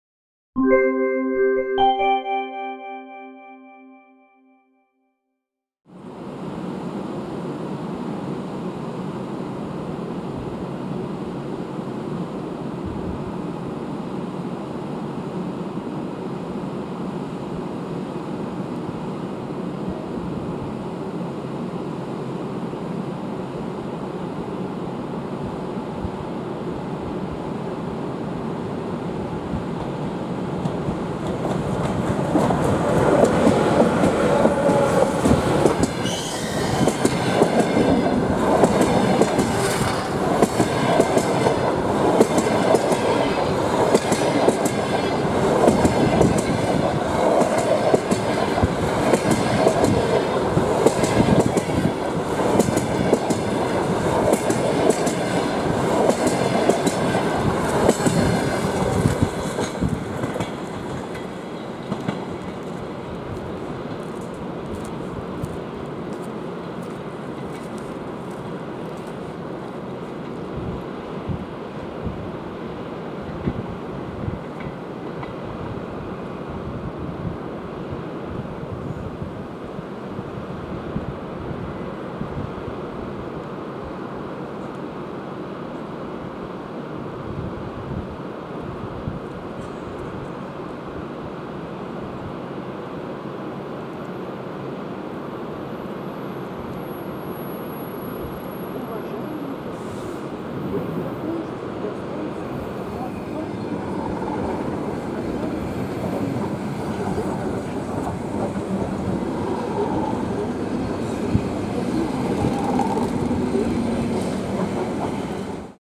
ЧС7-247. С поездом №376Я, сообщением: Москва - Воркута.
Снято: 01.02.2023, на станции Москва - 3, Ярославского направления.